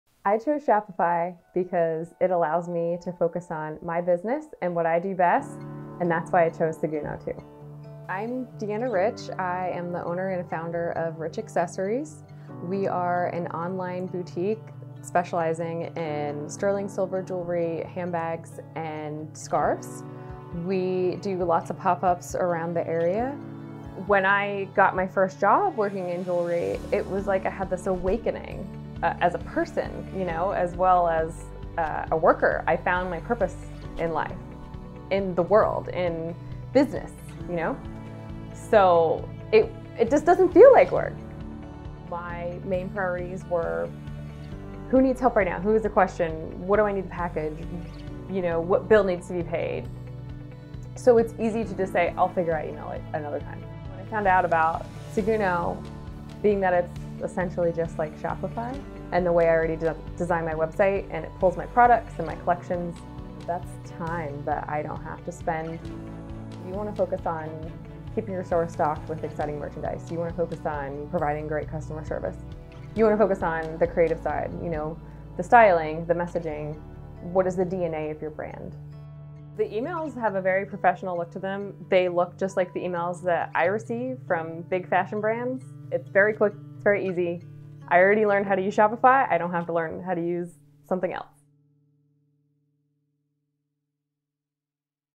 Seguno: Email Marketing Inside Shopify
Create, manage, and track your email marketing — without leaving Shopify.